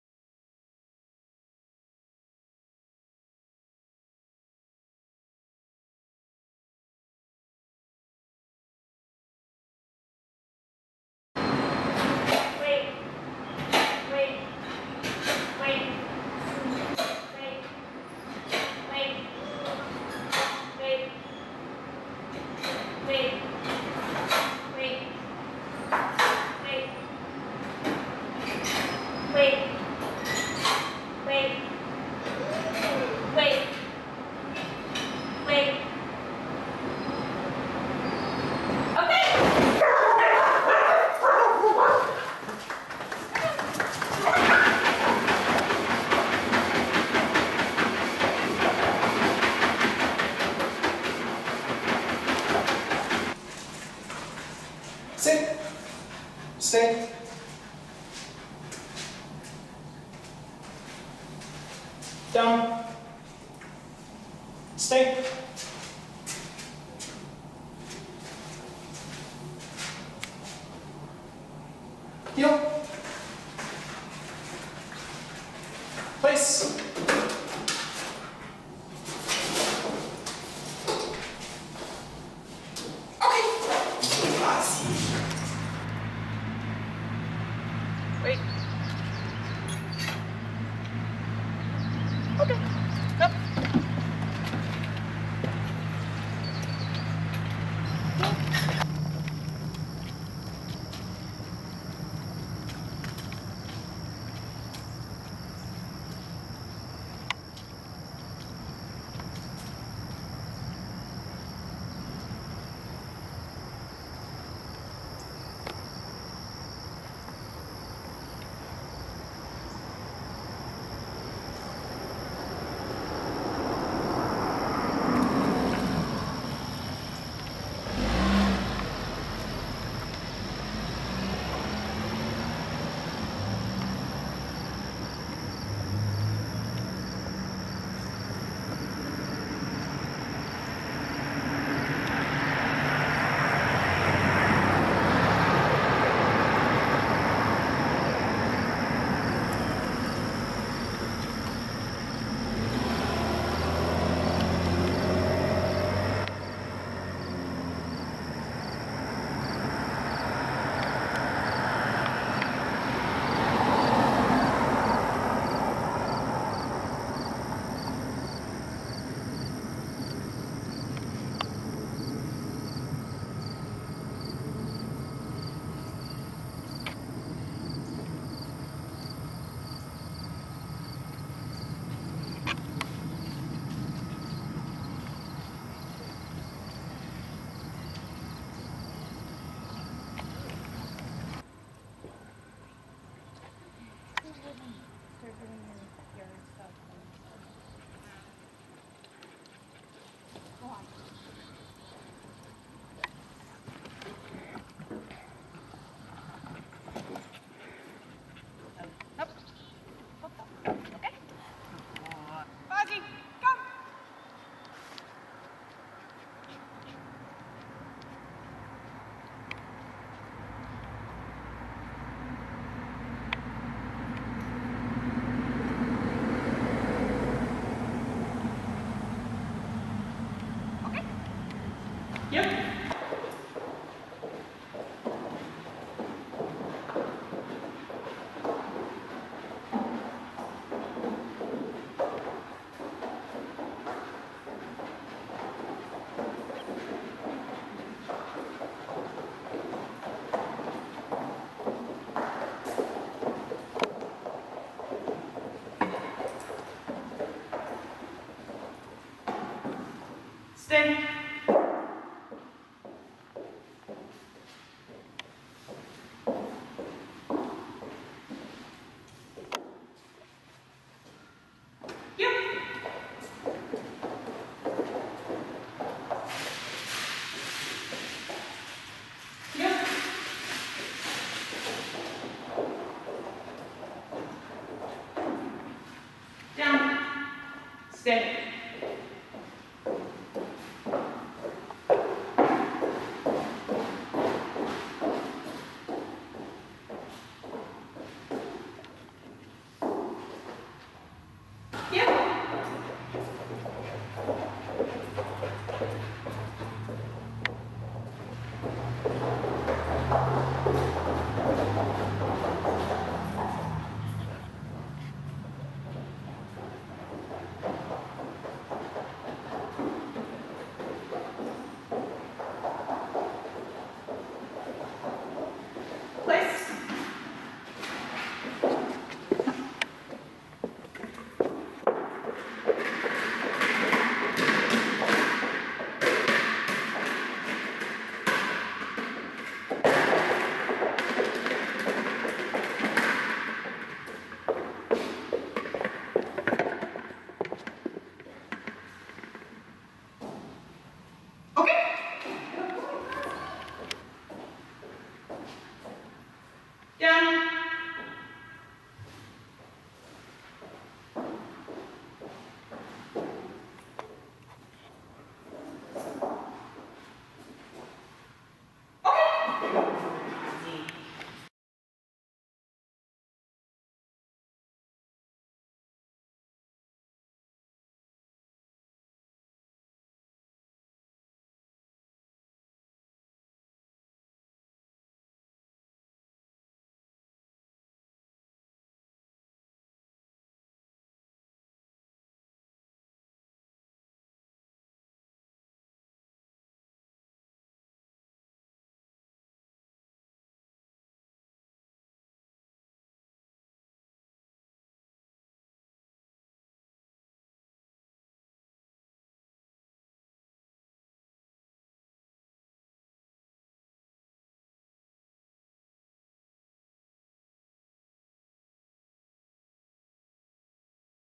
Fozzie (Irish Wheaten Terrier) Boot Camp Dog Training Video
Fozzie (Irish Wheaten Terrier) graduated from the dog training boot camp at Neuman K-9 Academy. This program included obedience commands to sit, stay, heel or walk on a loose leash, come when called, proper etiquette, no jumping up, meeting and greeting people under control, and running on a treadmill.

Our dog training camp provides programs for the Irish Wheaten Terrier such as boot camp, obedience training, and puppy camp.